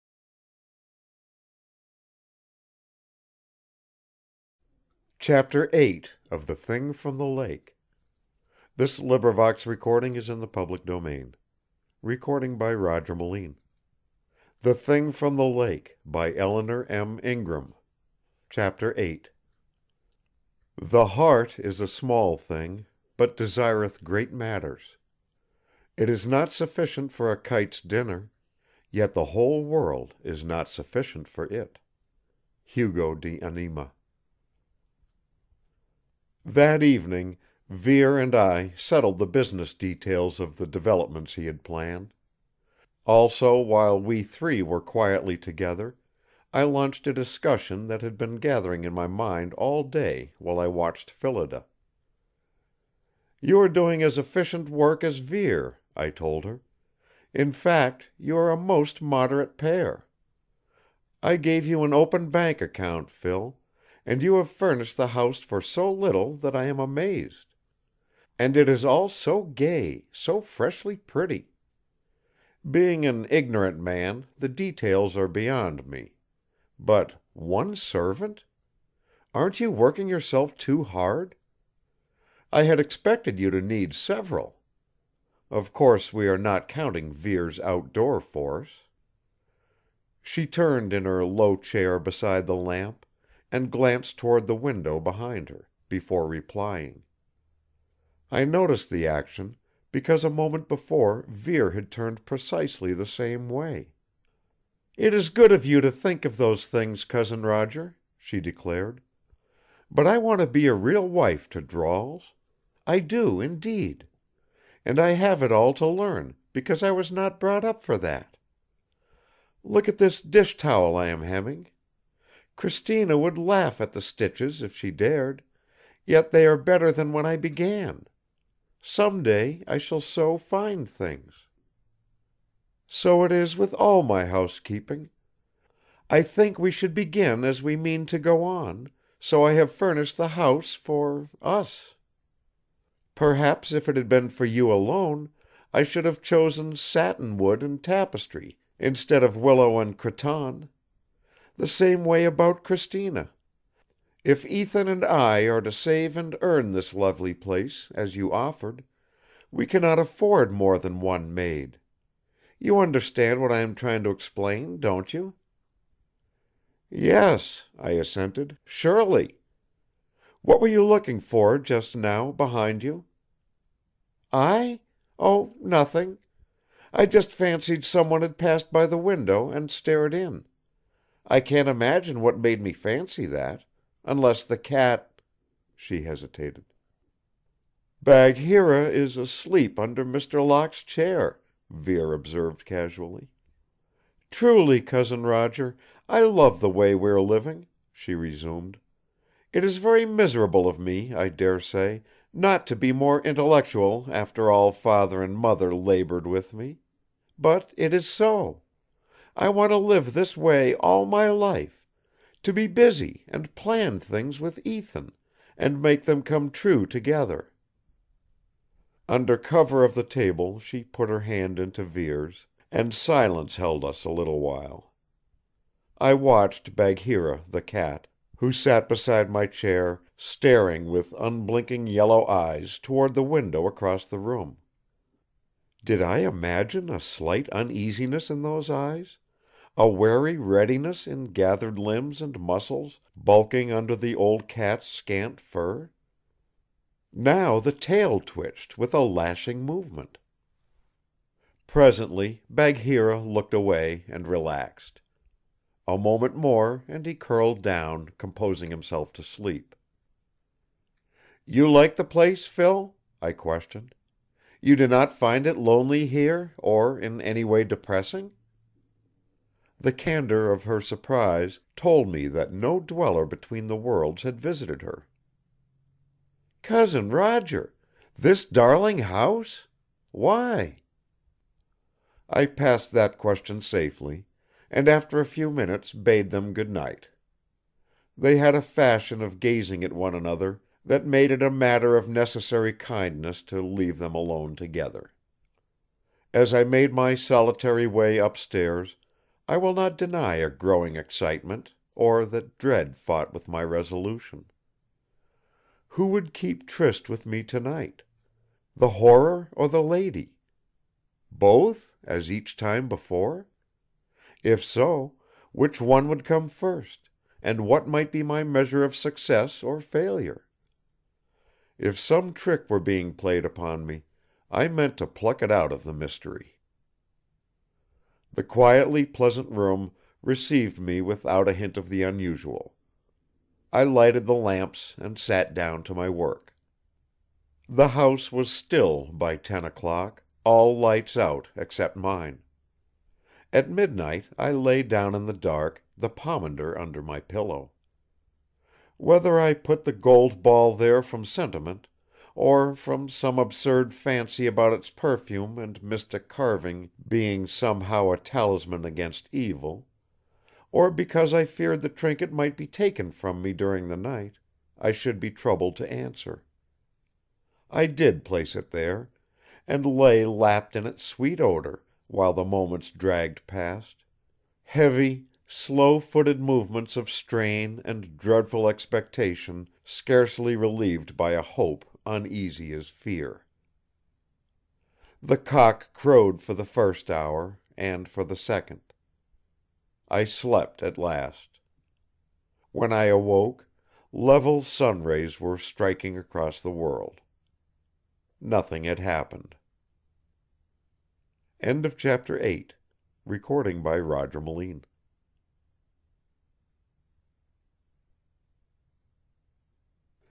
The Thing from the Lake by Eleanor M. Ingram - Chapter 8/22 (read by Roger Melin)
LibriVox recording of The Thing from the Lake, by Eleanor M. Ingram. Read by Roger Melin.